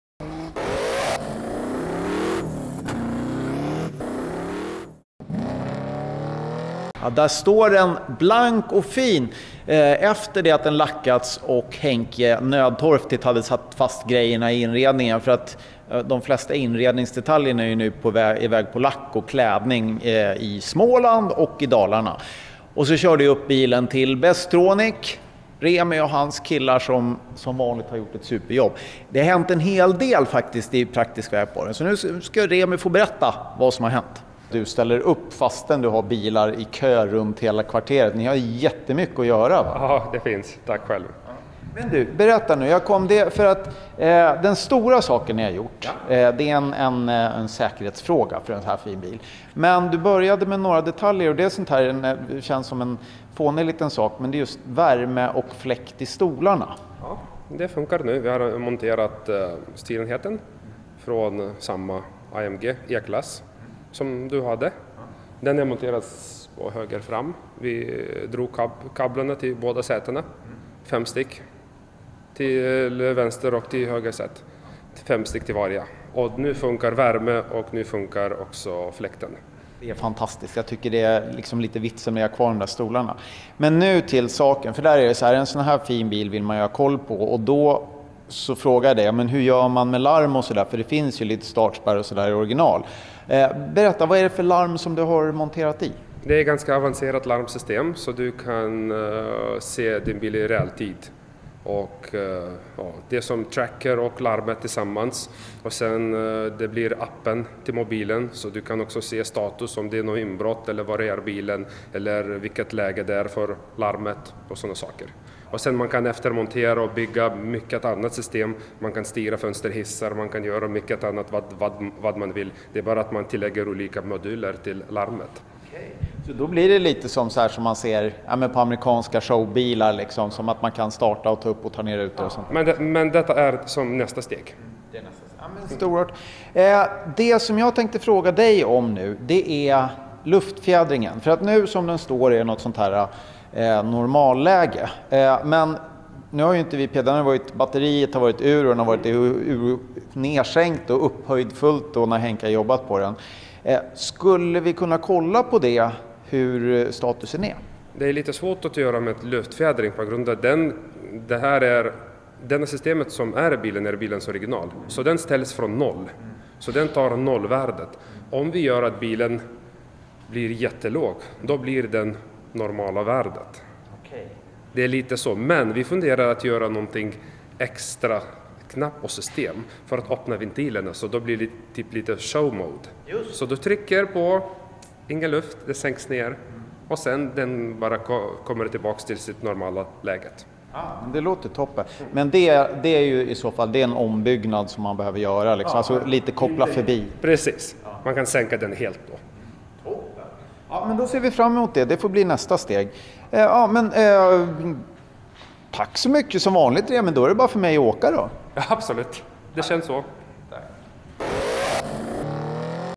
138. Larm, nivåfix och stols ac
I väntan på inredningsdetaljer från sadelmakare och träådrare kör Bobo Muskelmercan till Remi på Bestronic för att få larm monterat, luftfjädringen inställd och så förstås fixa värme och ac i stolarna. För Muskelmercan ska se lite äldre ut, men funktionerna ska vara toppmoderna!